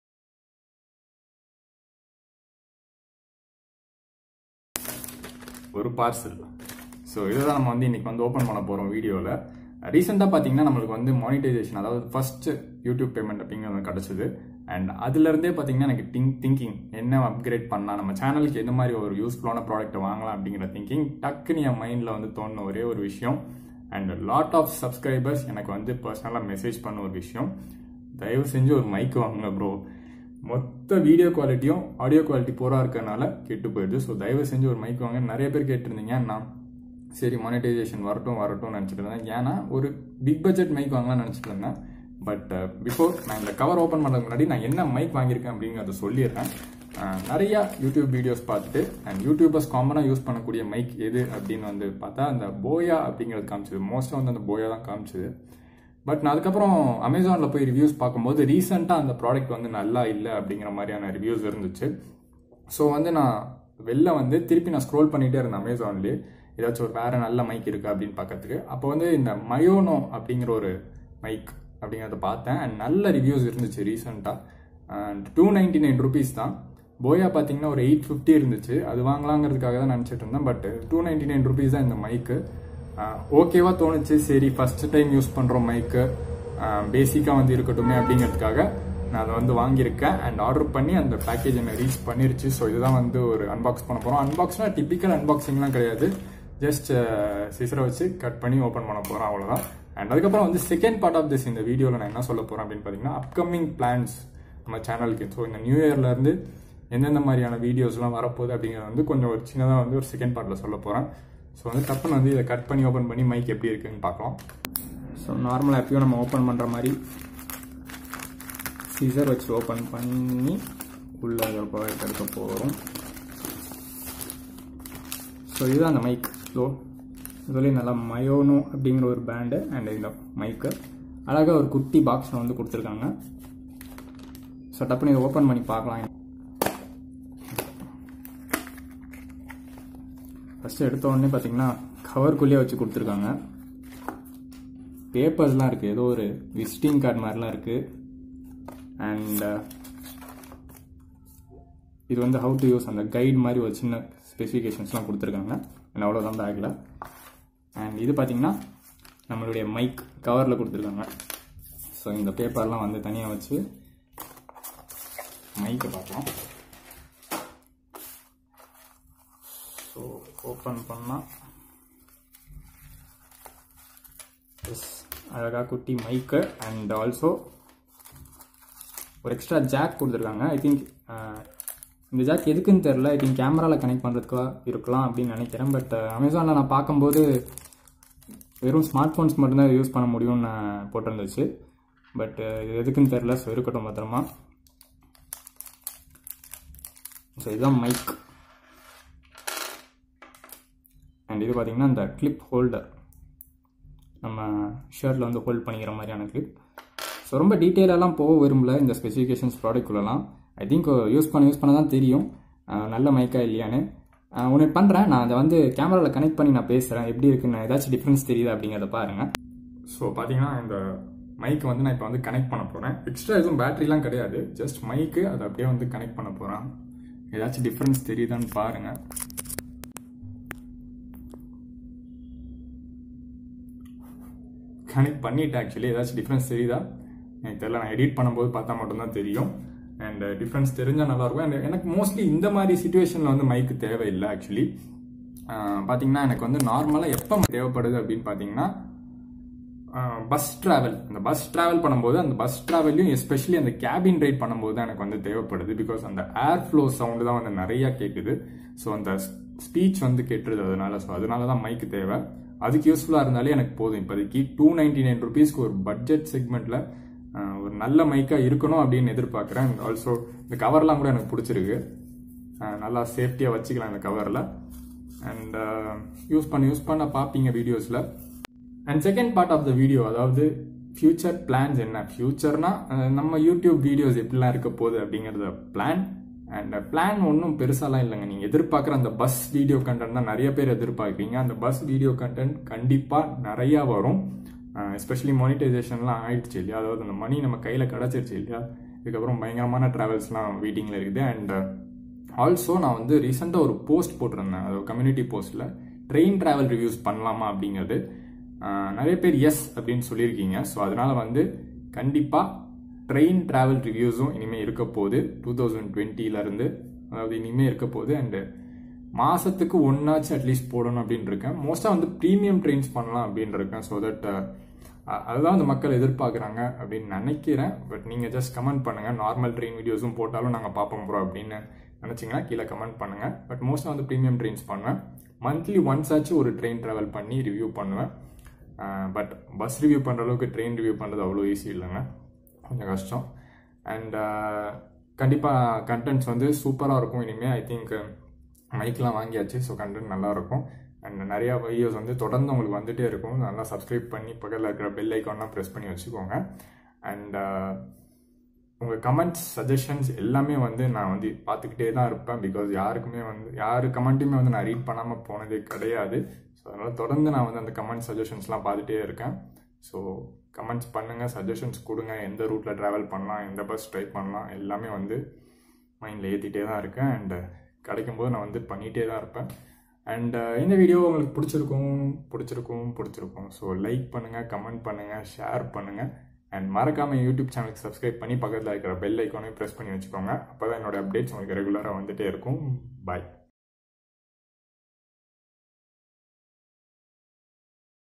Upgrading Videos with New Mic🎤|Maono microphone|Plans for upcoming vlogs🚂|Naveen kumar
Audio during travel review videos was a problem and to solve it a little,bought this basic microphone...also in this videos i updated about my upcoming vlogs...